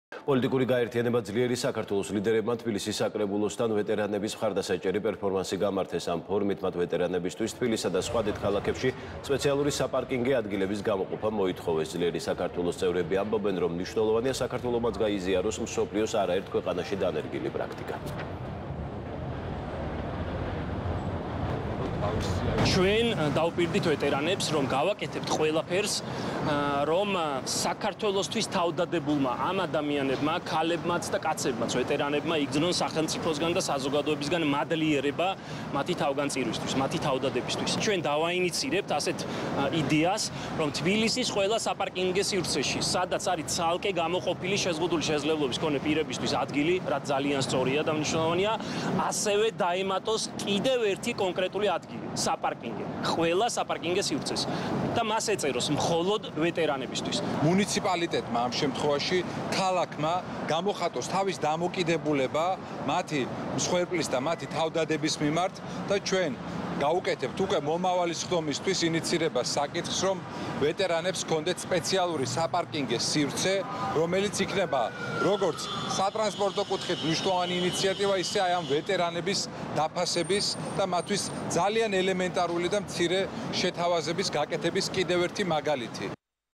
„ძლიერი საქართველოს“ ინიციატივა
პოლიტიკური გაერთიანება „ძლიერი საქართველოს“ ლიდერებმა თბილისის საკრებულოს წინ ვეტერანების მხარდასაჭერი პერფორმანსი გამართეს. ამ ფორმით მათ ვეტერანებისთვის თბილისსა და სხვა დიდ ქალაქებში სპეციალური საპარკინგე ადგილების გამოყოფა მოითხოვეს. „ძლიერი საქართველოს“ წევრები ამბობენ, რომ მნიშვნელოვანია საქართველომაც გაიზიაროს მსოფლიოს არაერთ ქვეყანაში დანერგილი პრაქტიკა.